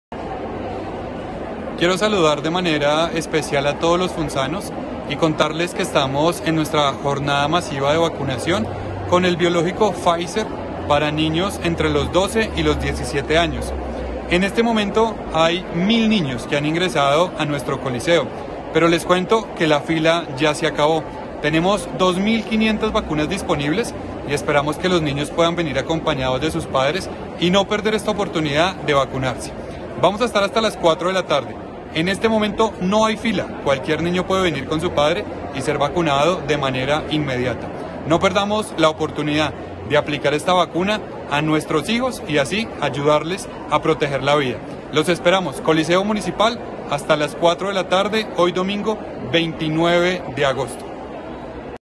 VACUNACIÓN NIÑOS EN FUNZA. DANIEL BERNAL, ALCALDE. 11.17 AM. COMUNICADO DE PRENSA LLEGA A LAS 11.14
*FUNZA APLICARÁ 2.500 DOSIS PARA JÓVENES ENTRE 12 Y 17 AÑOS*

_Hasta las 4:00 PM de este domingo, 29 de agosto, en el Coliseo de Funza se estará aplicando la vacuna Pfizer_

Personalmente, el alcalde de Funza, Daniel Bernal, lidera jornada de vacunación masiva para niños entre los 12 y 17 años, jornada en la que también se están aplicando, para niños entre los 1 y 11 años, las vacunas de rubéola y sarampión.

Al Coliseo Municipal ya ingresaron más de mil niños, pero lo cierto es que hay 2.500 vacunas listas para ser aplicadas el día de hoy.